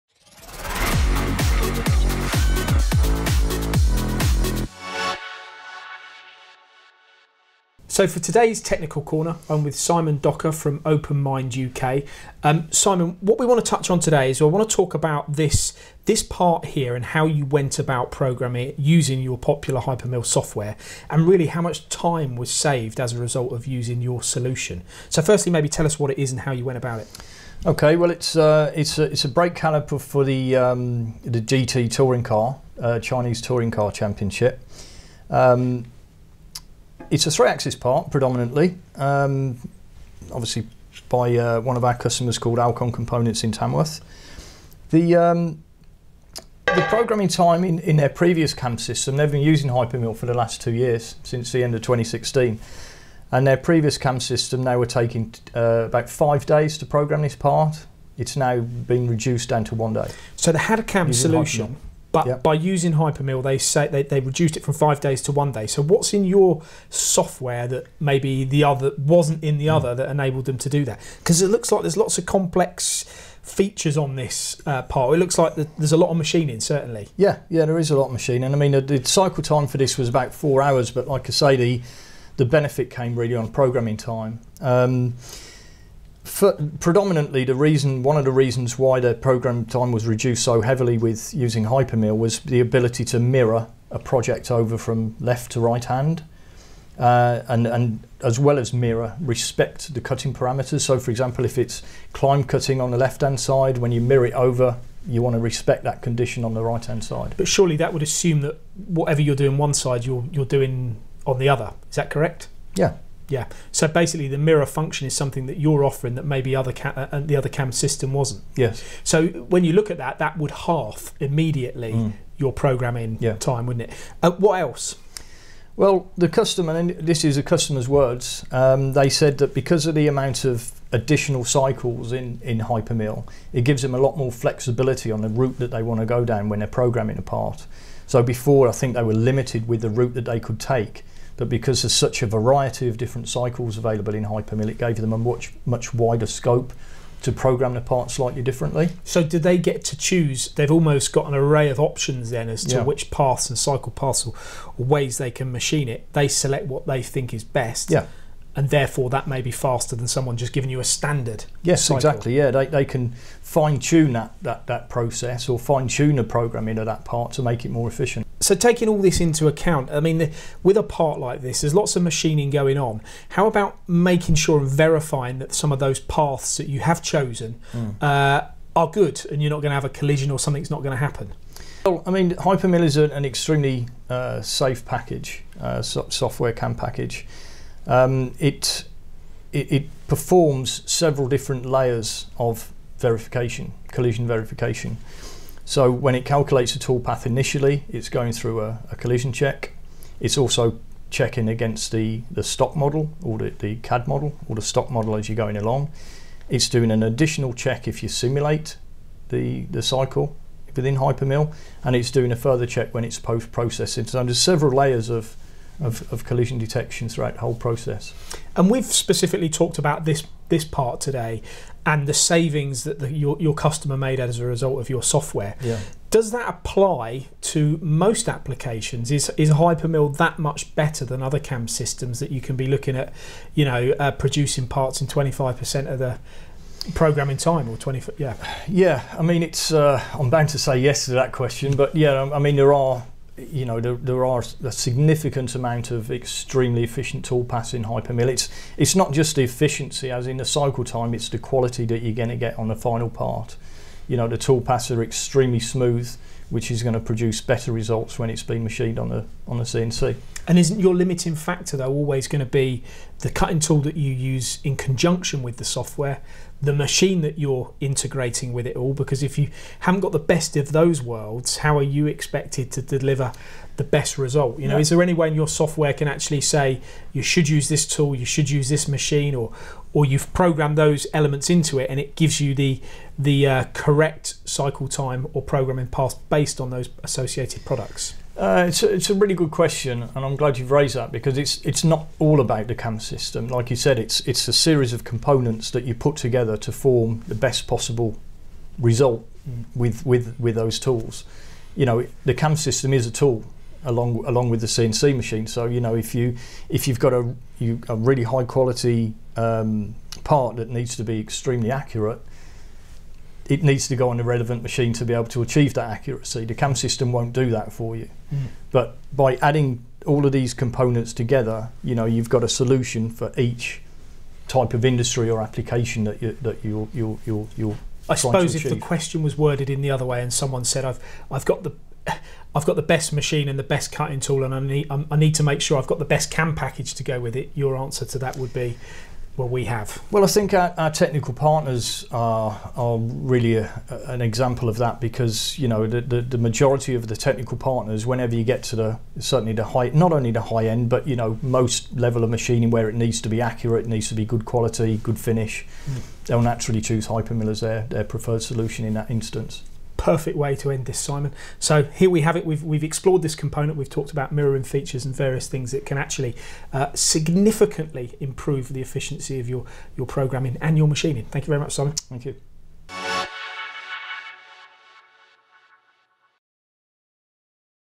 Is hyperMill the BEST CAM system?
Paul talks to Simon Docker from OPEN MIND about hyperMill software and its ability to outperform competitors CAM offerings on the majority of applications including the one in question here, a brake caliper.